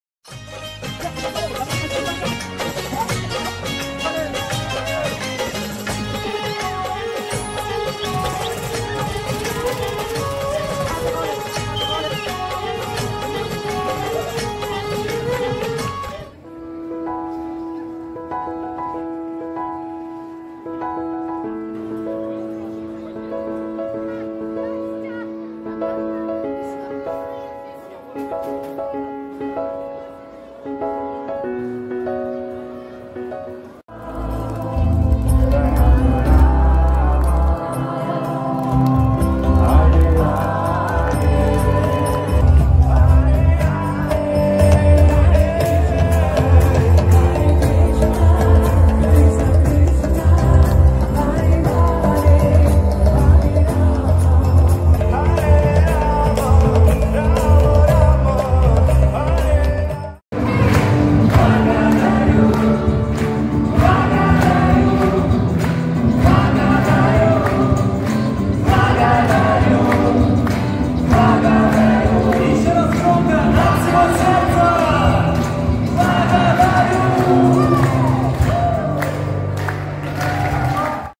Благость 2023
Коротенькое видео о прекрасных днях, проведенных в Анапе на фестивале Благость.
Каждый человек хотя бы раз в жизни должен побывать на этом фестивале, я думаю.
Это несравненно.